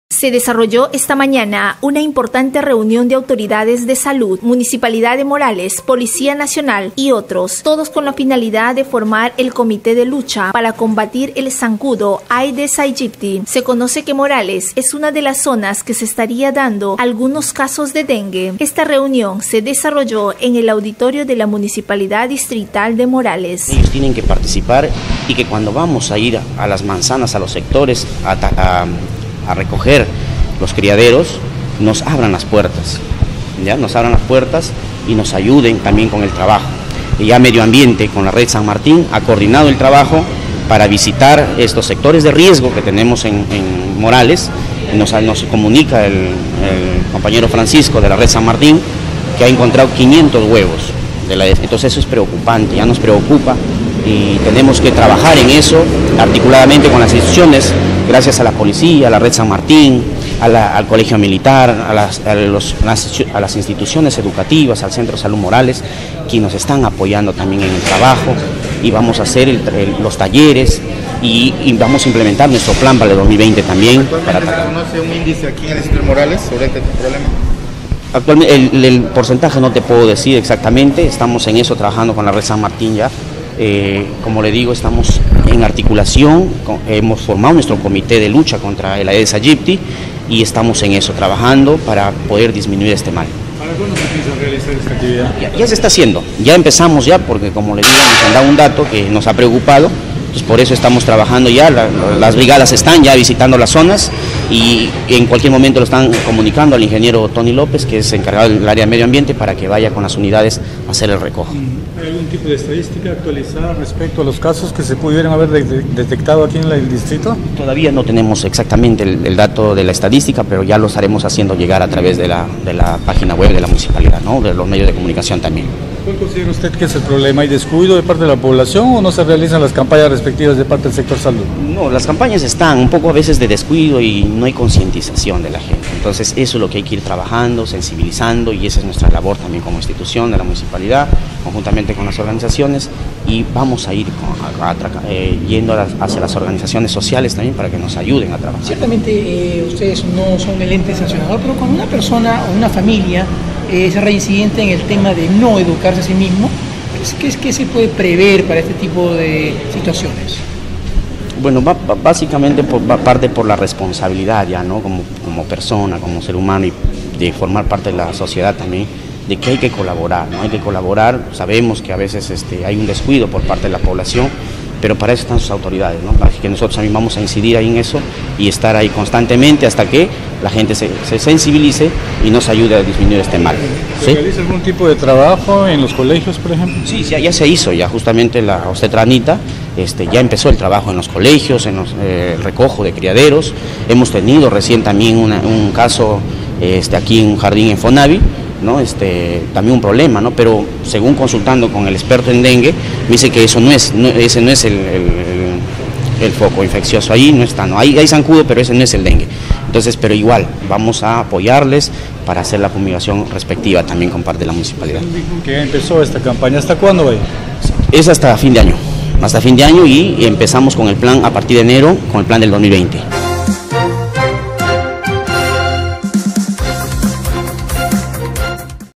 AUTORIDADES FORMAN COMITÉ DE LUCHA PARA COMBATIR EL ZANCUDO AEDES AEGYPTI
Se desarrolló esta mañana una importante reunión de autoridades de salud, muni de morales, policía nacional, y otros, todo con la finalidad de formar el comité de lucha para combatir el zancudo Aedes aegypti.  Se conoce que morales es una de las zonas que se estaría dando algunos casos de dengue. Esta reunión se desarrolló en el auditorio de la muni de morales.
                        Entrevista---Alan Saavedra becerra---responsable de desarrollo social de la muni de morales.